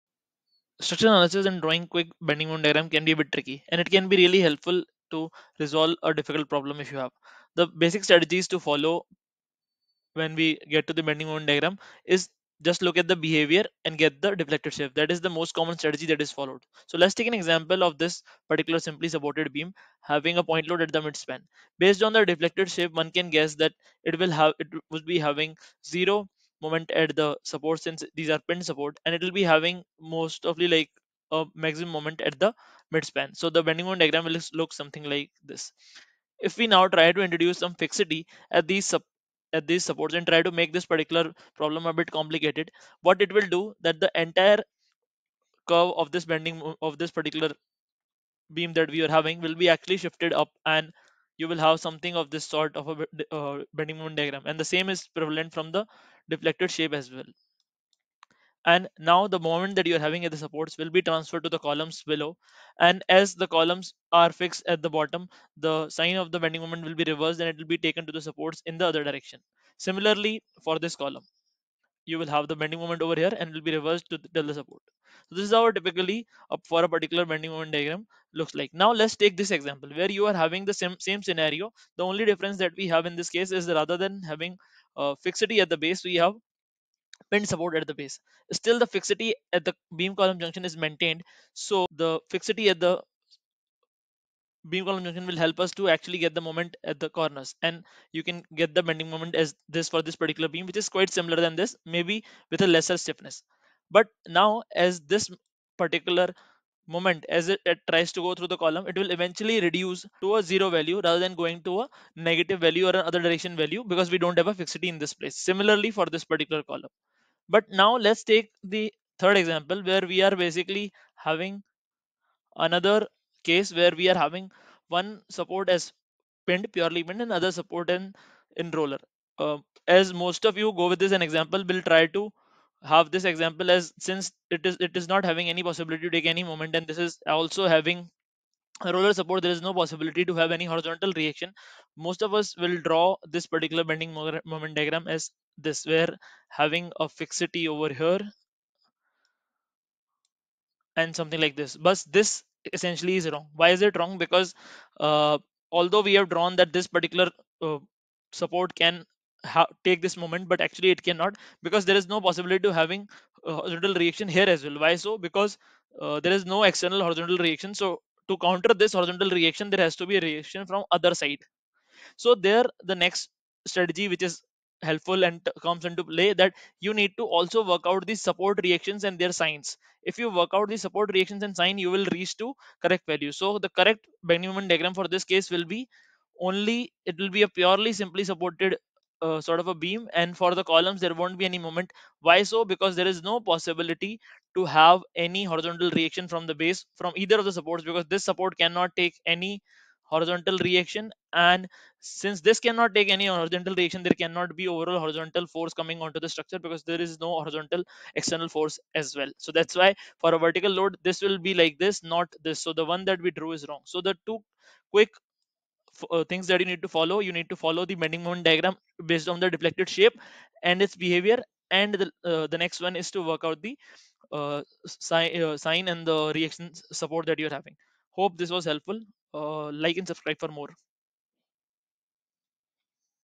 Structural Analysis Hacks 2
This video describes the hacks to follow for structural analysis and drawing BMDs.